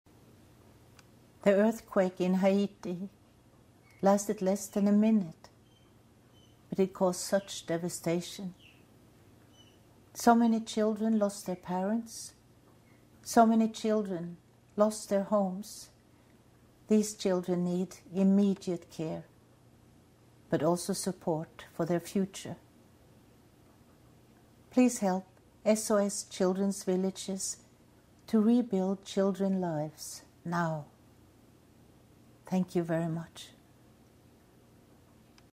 Haiti earthquake: Appeal of Liv Ullmann
SOS Children's Villages is supporting thousands of quake-affected children in Haiti. Our international donation gateway for Haiti can be found At the moment, this country is going through terrible times and we would like SOS Children's Villages to increase their help not only in certain areas, but to extend it to the whole country. There are areas that have been very badly hit, very badly, such as the downtown area and others. People there have nothing, nothing, the houses are completely destroyed! SOS Children's Villages is collaborating with other organisations to provide the people with what they need, not just the children but also parents and Haitians in general. All Haitians are equal now, because nobody has anything, there are no rich people and no poor people, everybody sleeps outside. We want to thank SOS Children's Villages for their efforts to improve our situation and the situation of Haiti.